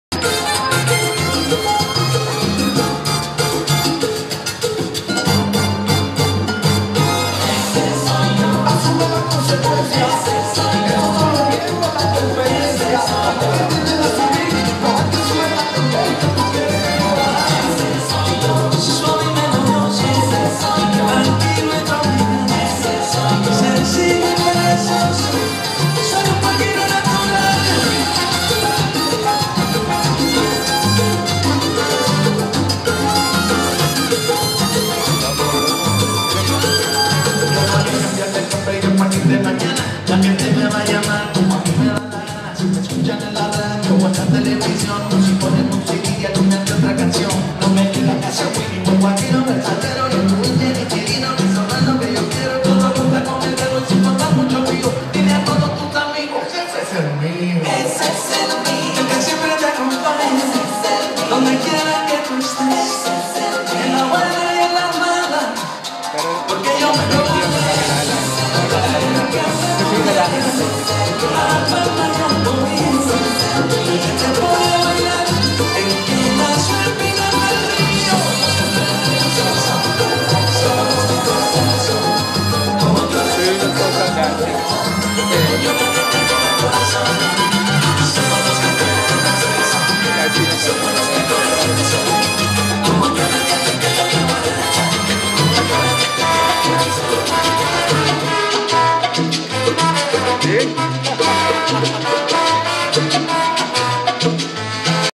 Salsa Nights with Sal
Our group enjoying the night at our social dancing class.